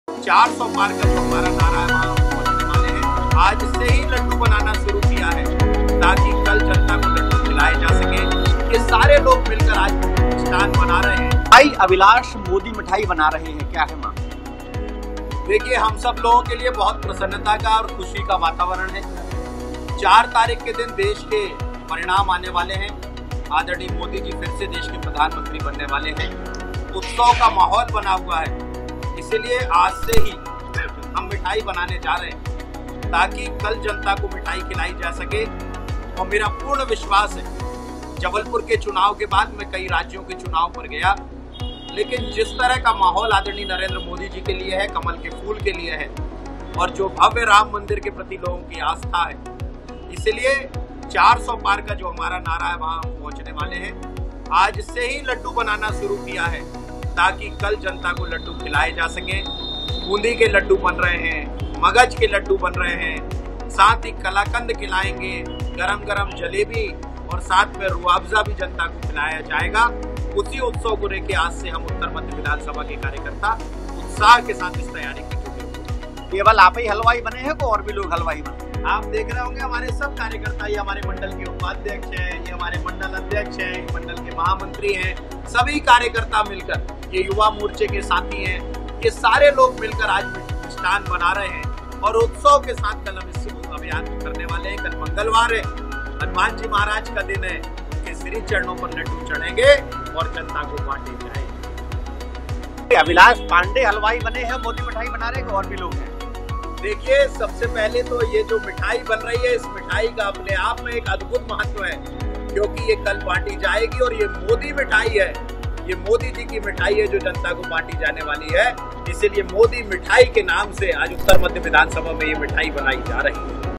Lok Sabha Election Results 2024: जीत से पहले Jabalpur में BJP ने शुरू की जश्न की तैयारी | MP News
Lok Sabha Election Results 2024: जीत से पहले Jabalpur में BJP ने शुरू की जश्न की तैयारी | Madhya Pradesh | MP News

'अबकी बार 400 पार' का (Loksabha Election 2024 Results)नारा देने वाली बीजेपी एग्जिट पोल के (Exit Poll 2024)बाद और भी जोश में आ गई है. 4 जून को नतीजे (Loksabha Elections)अपने पक्ष में आने की संभावना को देखते हुए (Loksabha Election Results) बीजेपी अभी से जश्न के मूड में आ गई है. जी हां, एग्जिट पोल (Loksabha Elections Exit Poll)के नतीजों के बाद बीजेपी अब बड़े जश्न की तैयारी में जुट गई है.